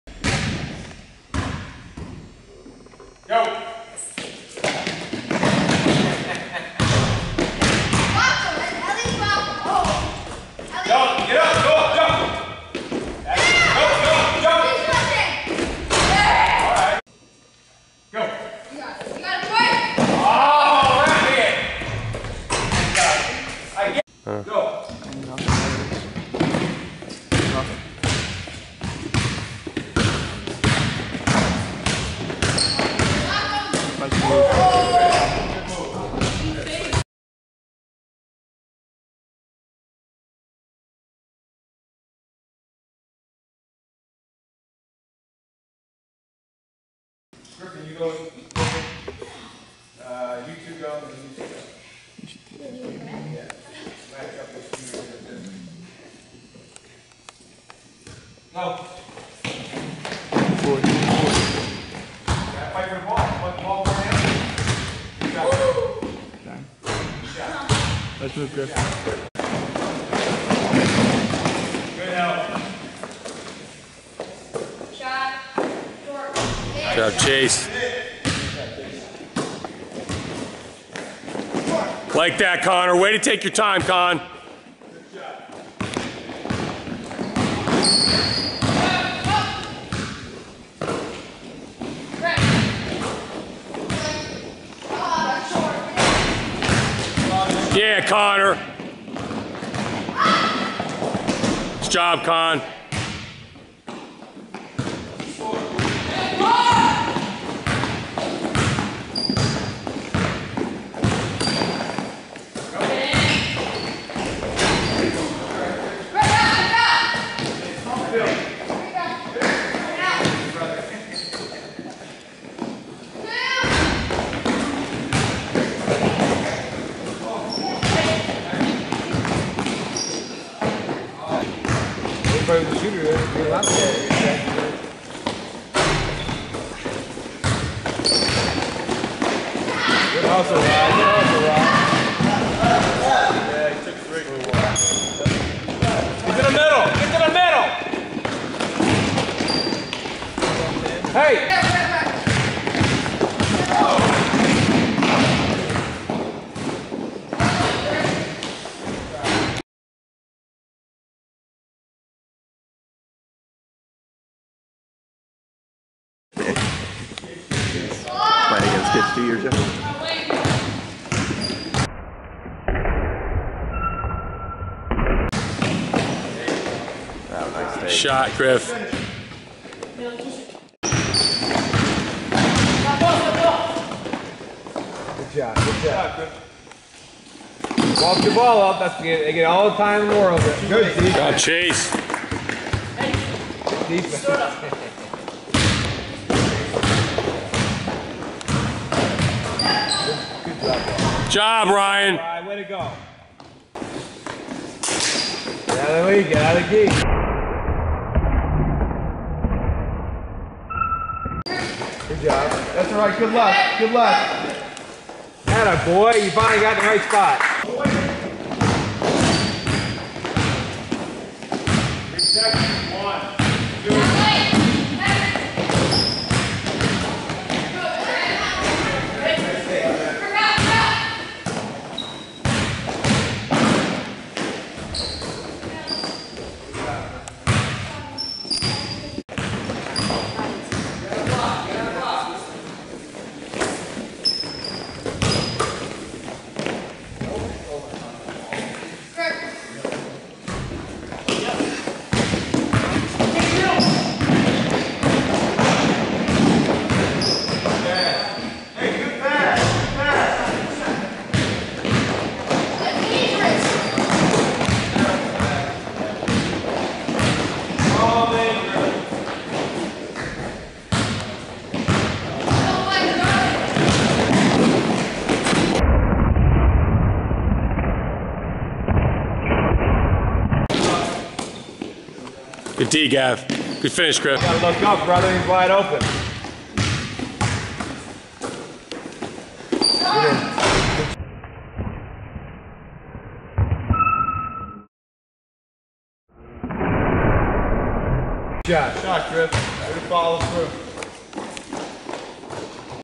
Nov 18th, 2011 Friday Night Practice - Griffin Murphy (2017)
Griff''s brother Chase played in this 4 on 4 scrimmage!  He hustled and was fearless!  RBA 13U practice/scrimmage!